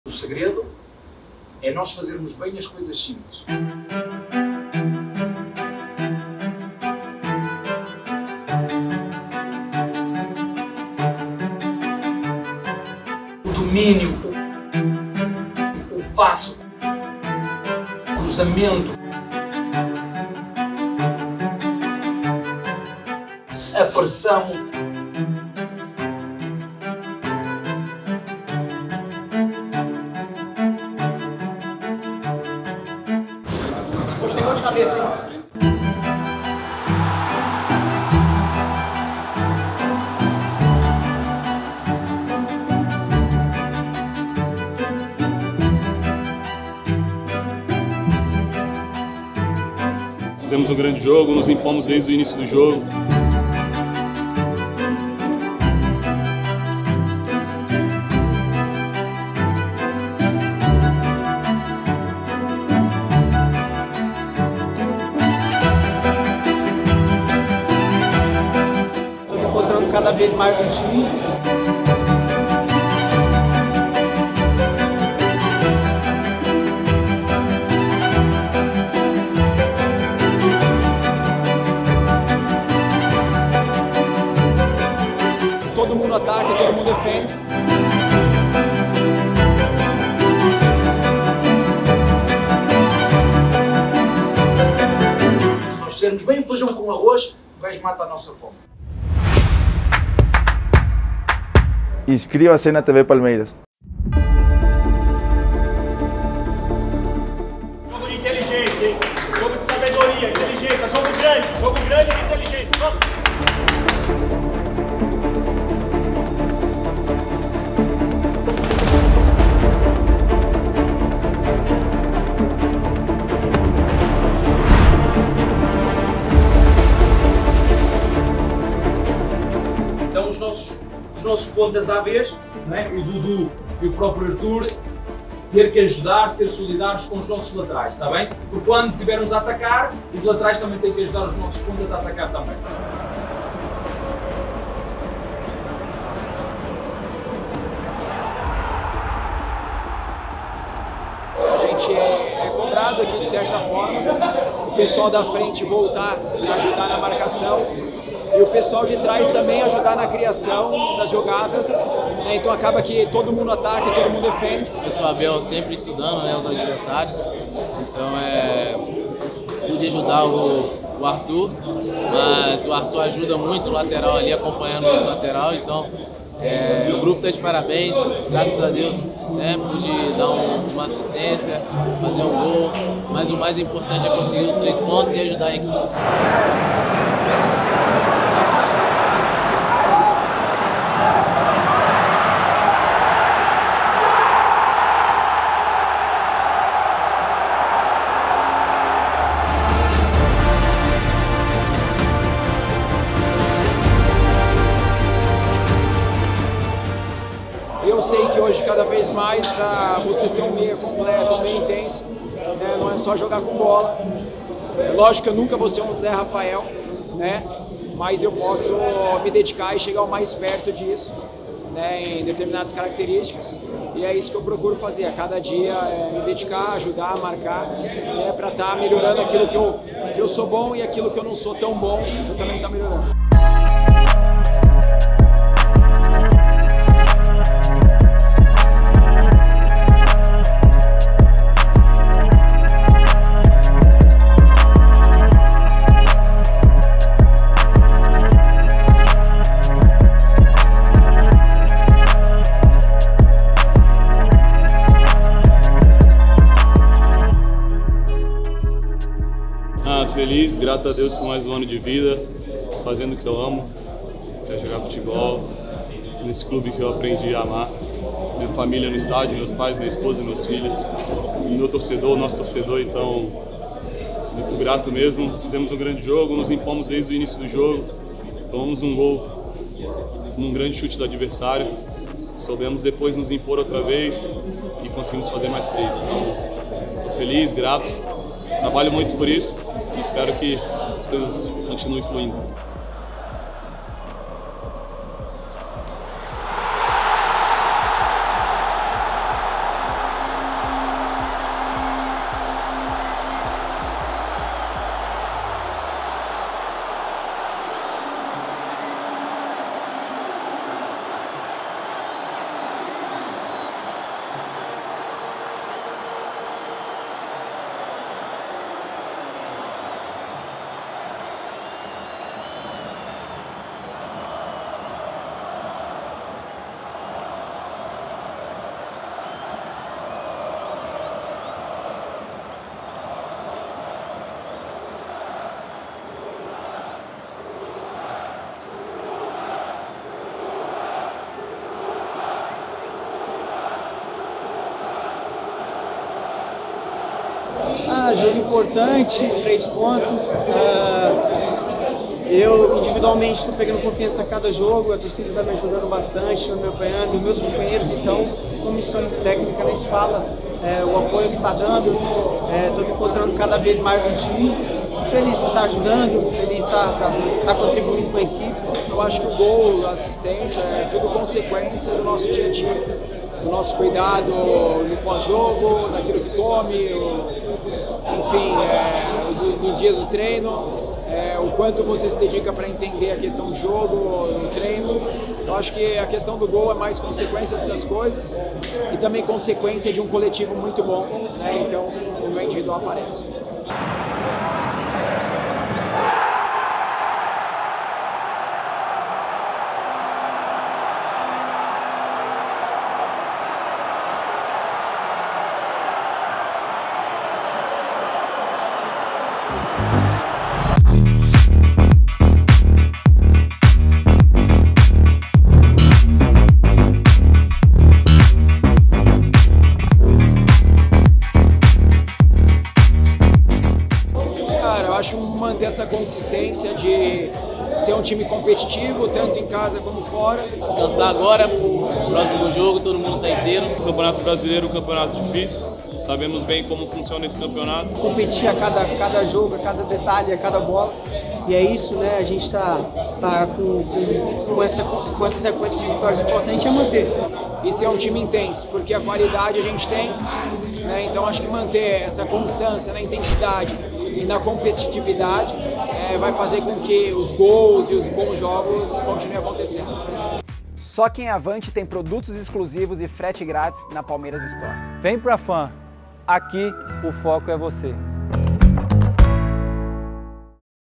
BASTIDORES | PALMEIRAS 4 X 1 GRÊMIO | CAMPEONATO BRASILEIRO 2023
Confira todos os detalhes da vitória do Verdão sobre o Grêmio no Campeonato Brasileiro.